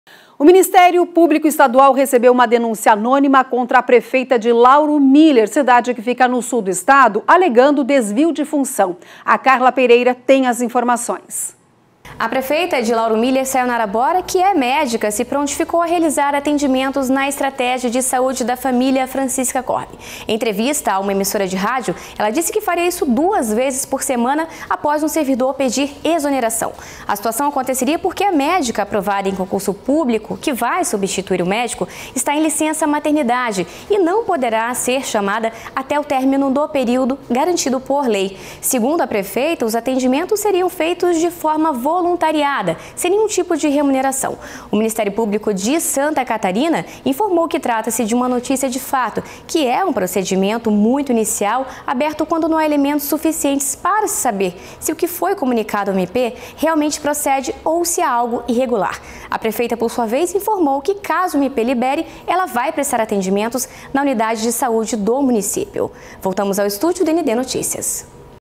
Prefeita de Lauro Müller tenta atender em posto de saúde, mas é impedida por denúncia
O Ministério Público Estadual recebeu uma denuncia anônima contra a prefeita de Lauro Müller, no Sul do Estado, alegando desvio de função.

Confira.